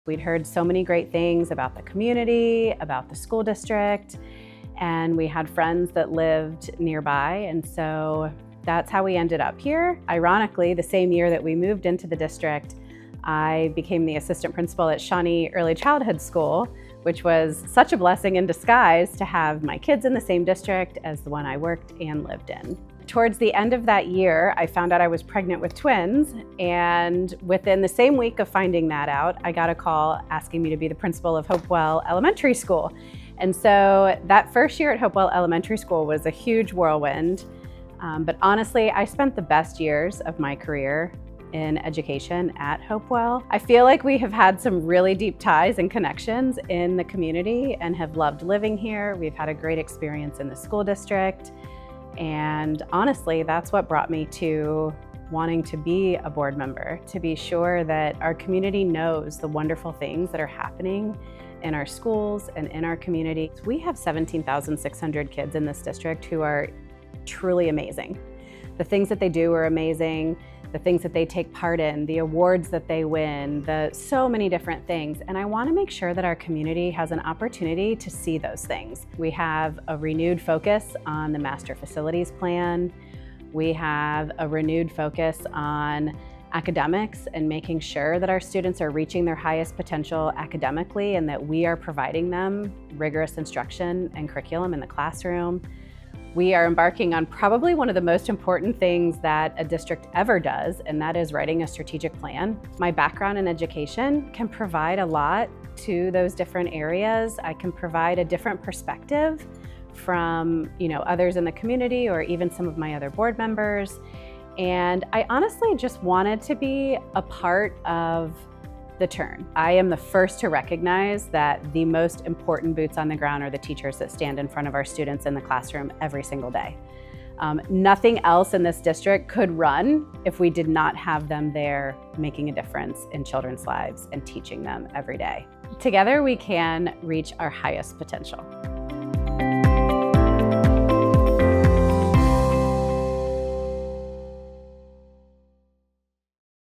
Meet the School Board: Christina French
It's time to meet another Lakota Board of Education member!

Christina French is a former Lakota administrator and current parent in our district. She reflects on how her best years in education were spent right here in Lakota and how that experience has brought her to the Board. She's all in for our students and staff, working together to reach our highest potential.